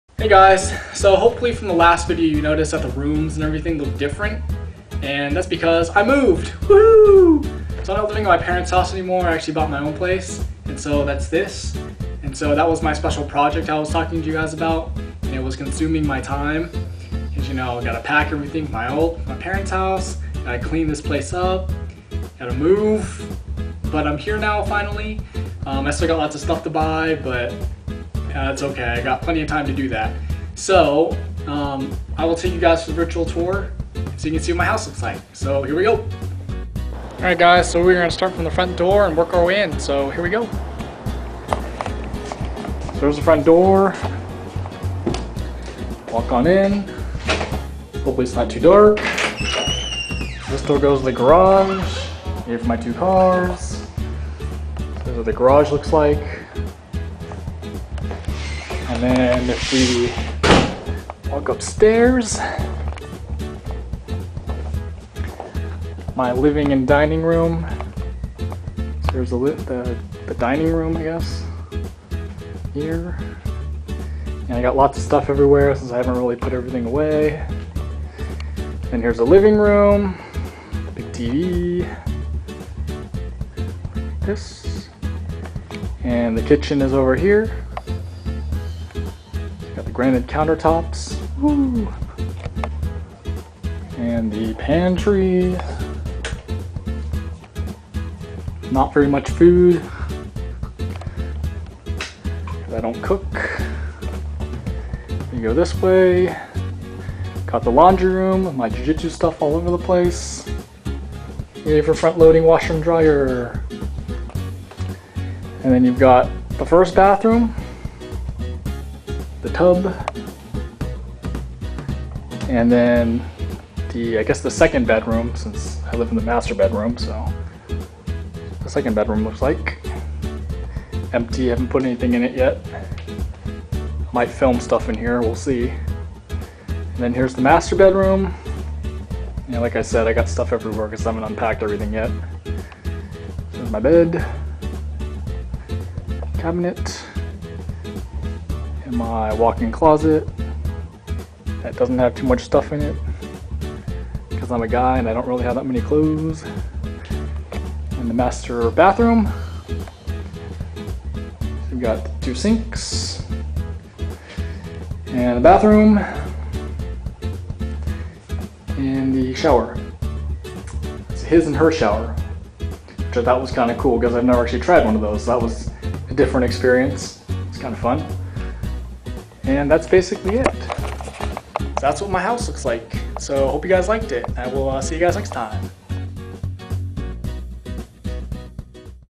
Special Project done! My new house!
So I finally moved into MY house.  Hopefully now I'll have a little more time to make videos for you awesome youtubers!

Just thought I'd let you know that in 2 weeks, I'll be headed to Japan for 2 weeks, so I'll get some good footage there and show you guys what I did over in the Far East